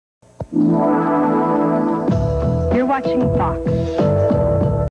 KARD Station ID 1996 #1
This is KARD's station ID from 1996
KARD is located in West Monroe, LA